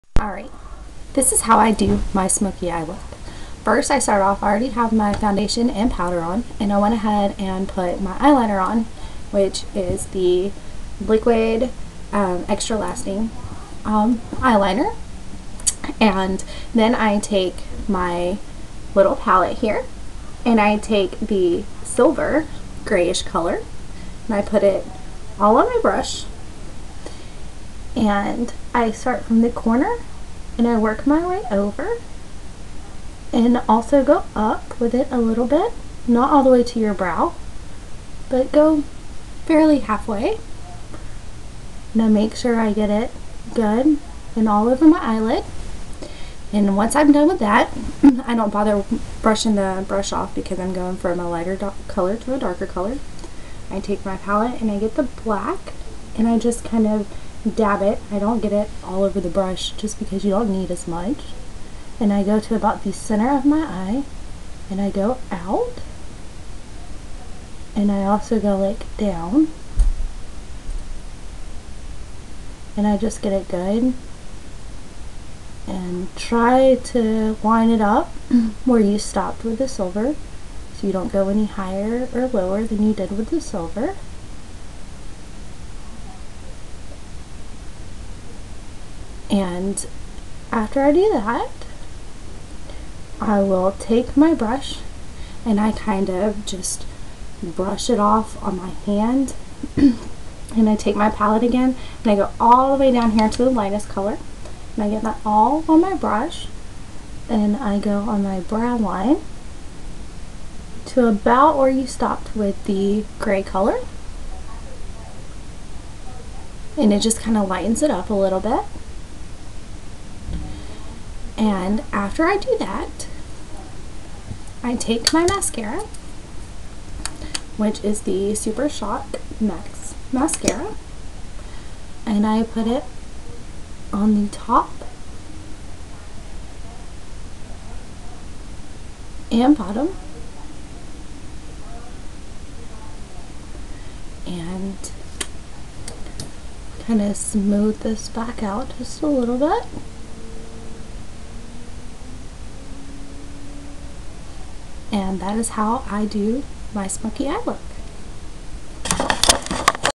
Avon Makeup Maven Southeast: Amanda B.
Amanda B. created a Eye Look called smokey eye look using Extra Lasting black eyeliner
super shock max mascara black
eight in one eye palette (watercolors) colors used were silver, black, and light peach
ideal flawless foundation in nude
ideal flawless powder in light medium